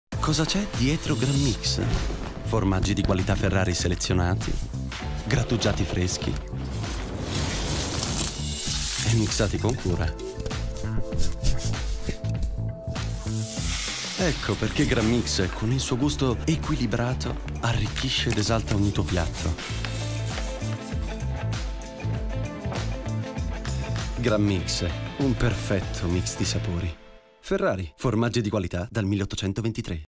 Ferrari G. riporta Grand Mix in comunicazione e punta sui mercati esteri
La storica industria casearia di Lodi rilancia il prodotto di punta a base di formaggi grattugiati freschi col packaging rinnovato da Cba e la campagna in tv e sul web il ‘mixologo’. Ideata da Leo Burnett, fa un parallelo tra l'expertise di Ferrari e quella dei bartender nel raggiungere il mix perfetto. Il budget è tra 1,5 e 2 milioni. L'azienda ha fatturato 120 milioni nel 2018, valore su cui GranMix incide circa per un terzo. L'export pesa per ora meno del 20%. (3 aprile 2019)